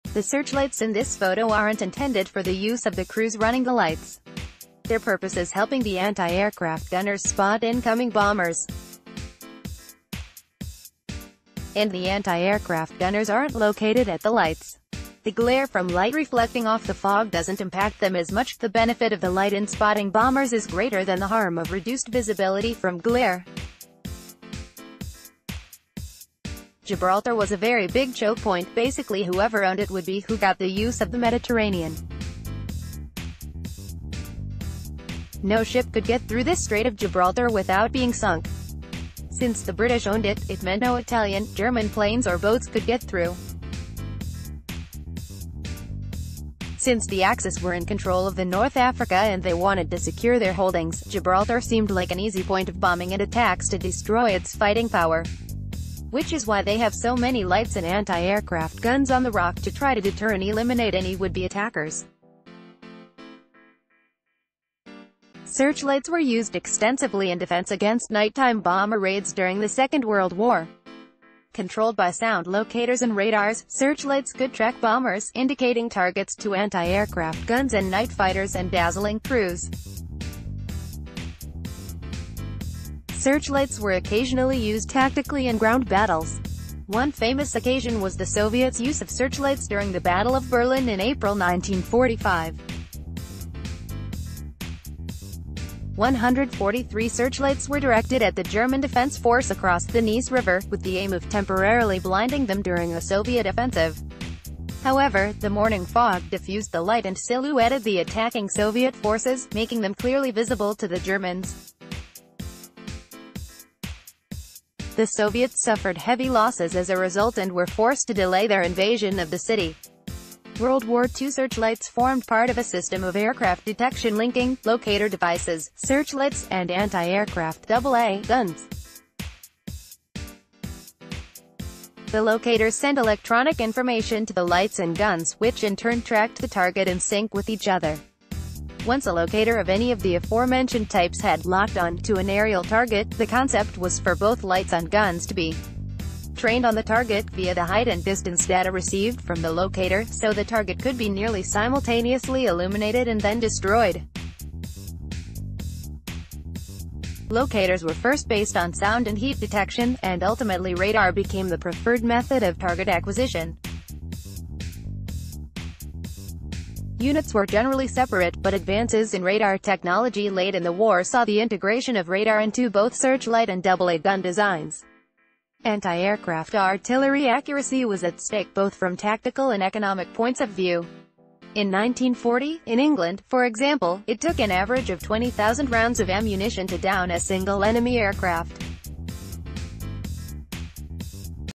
Searchlights on the Rock of Gibraltar, 1942
The searchlights in this photo aren’t intended for the use of the crews running the lights. Their pu...